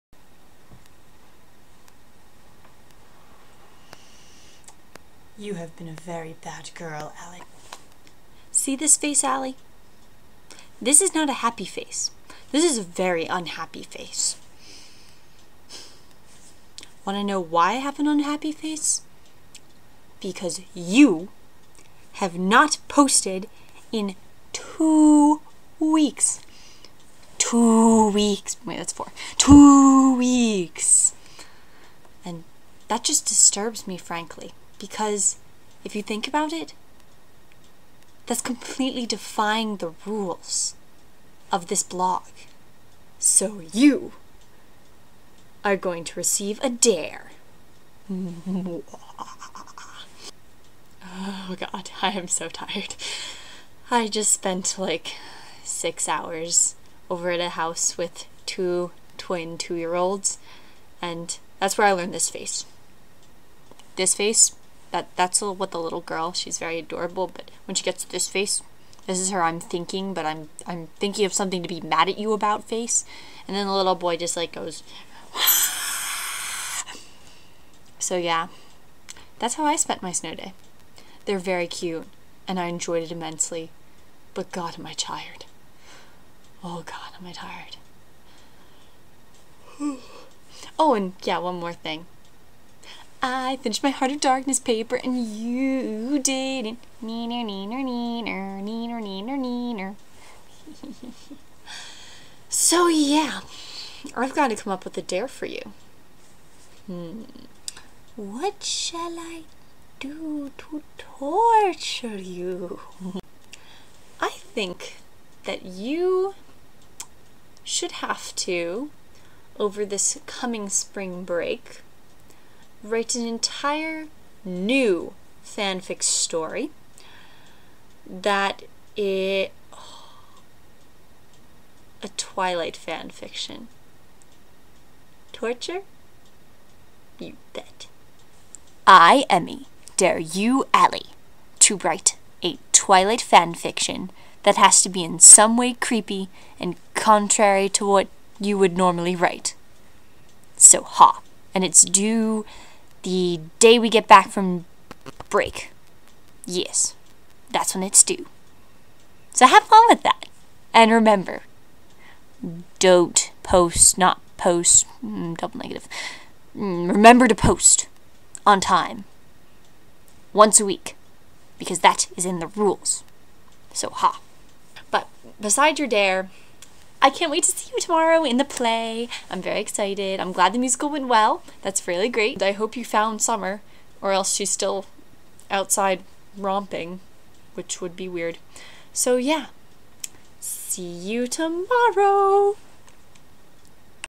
DARE
Evil laughs, creepy face stroking, and exhaustion, with a bit of daring on the side.  HEHEHE